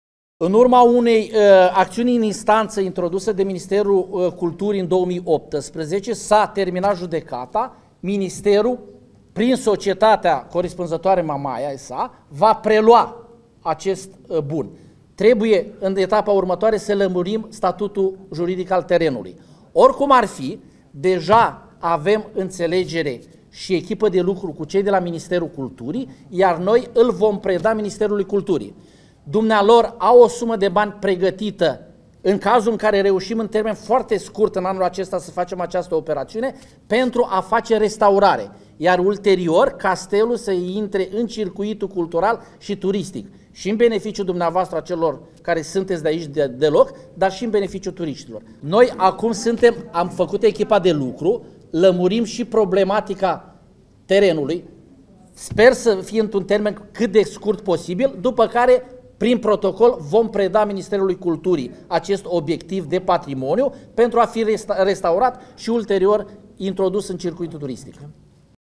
Cadariu | sper să intre în circuitul public
Ministrul Antreprenoriatului și Turismului, Daniel Cadariu, a vizitat astăzi și Palatul Reginei Maria. De aproape un secol, în centrul staţiunii Mamaia tronează fosta reşedinţă de vară a Reginei Maria, locul pe care aceasta l-a adorat. Lucrările măreţei reşedinţe de vară au început în 1923 şi s-au finalizat trei ani mai târziu. „Este o înțelegere cu Ministerul Culturii să preia castelul. Și terenul de sub castel  care va investi pentru restaurare și reintrodus în circuitul turistic”, a afirmat astăzi la Constanța, ministrul turismului. Aproximativ 2 milioane de euro vor fi alocați pentru restaurare.